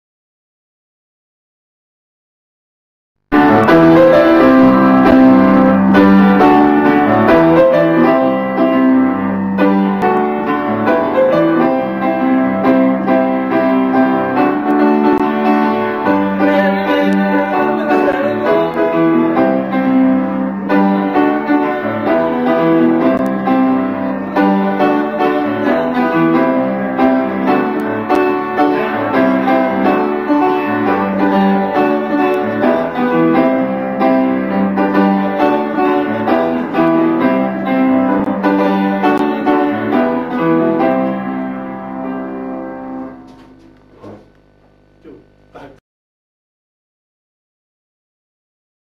Mixian video2
Mixian si diletta a cantare una canzone della sua autrice preferita (all'epoca)